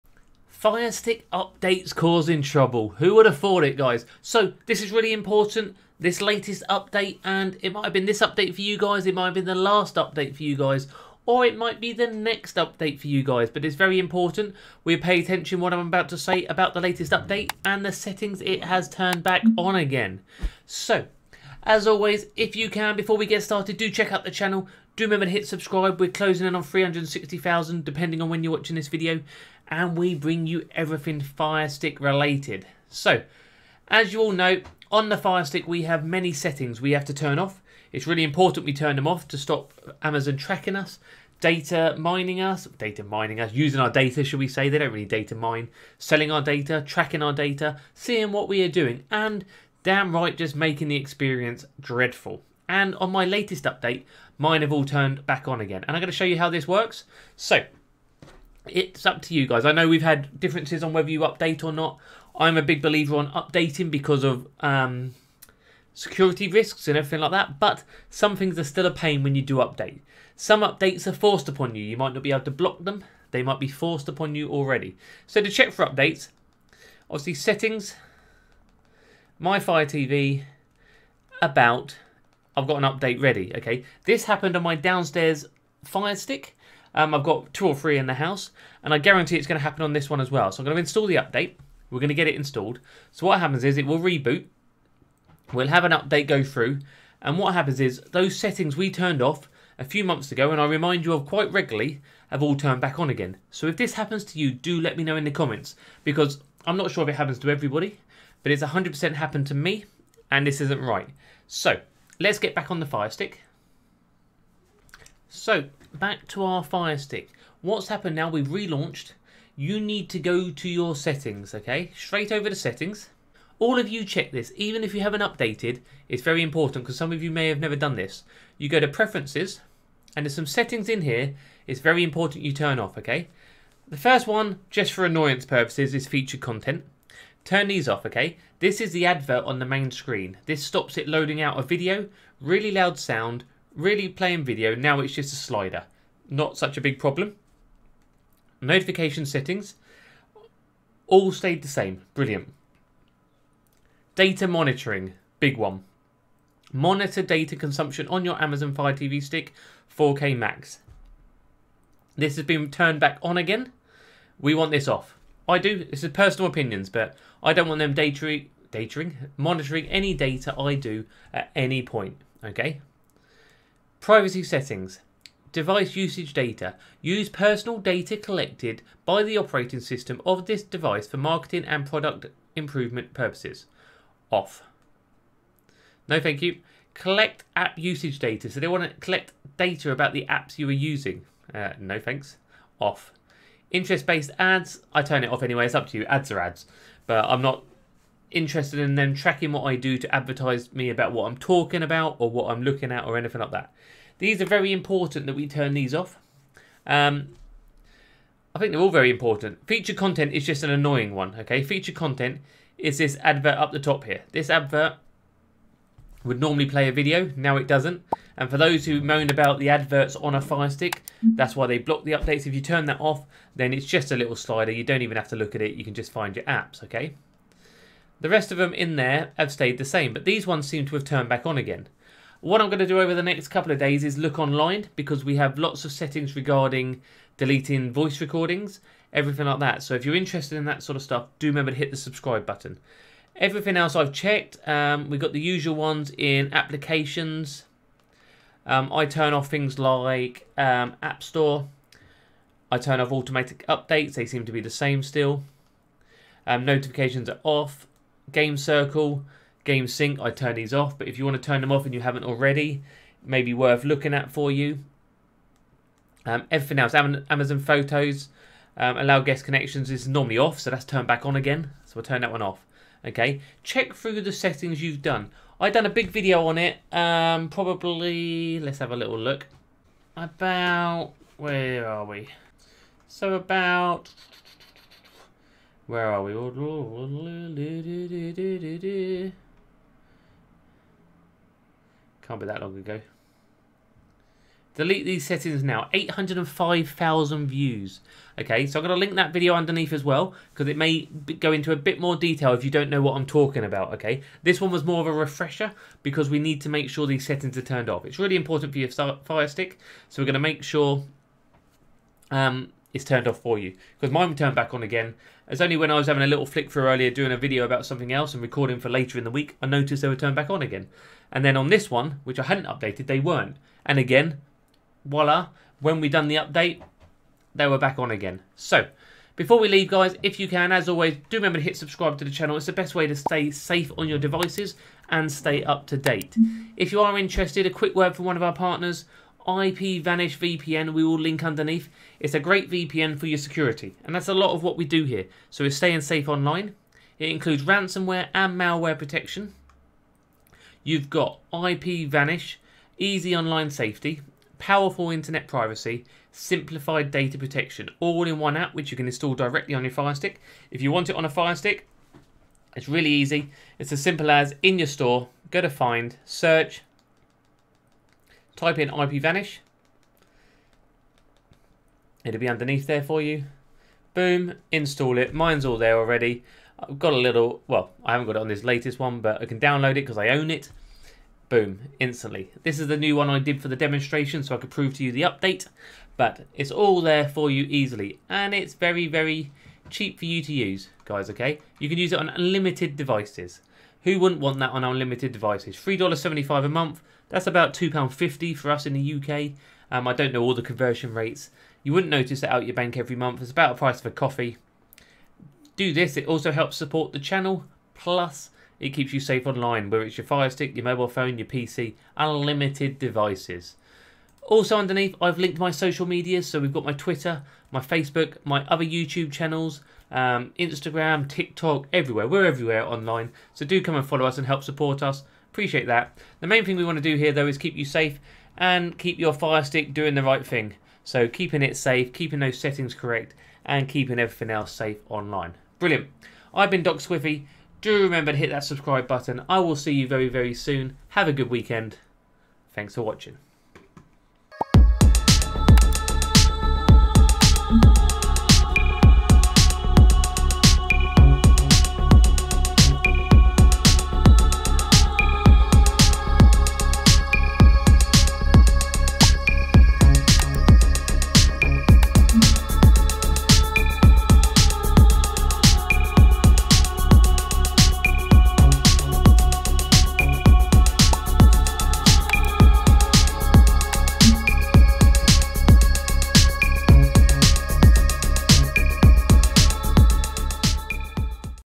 WARNING FIRESTICK Update! SETTINGS YOU NEED TO TURN OFF NOW!!! 2022 !
Firesticks and Fire tv are great free streaming devices, But keeping them buffer free and running safely is key, You may of changed these settings but updating your firestick can change this!